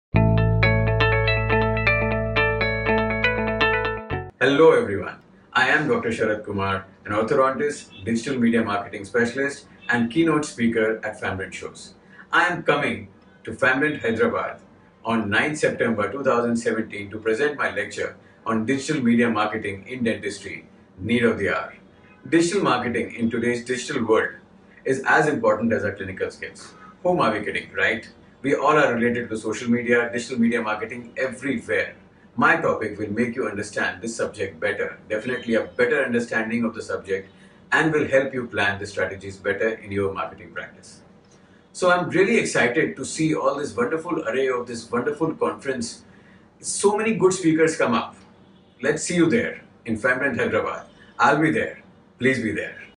Famdent Show Hyderabad 2017 Dr. Sharad Kumar Keynote Speaker
Promotional video of lecture of Dr. Sharad Kumar on Digital Media Marketing for Dentists: Need of the Hour, in upcoming Famdent Show in Hyderabad 9-10 September 2017.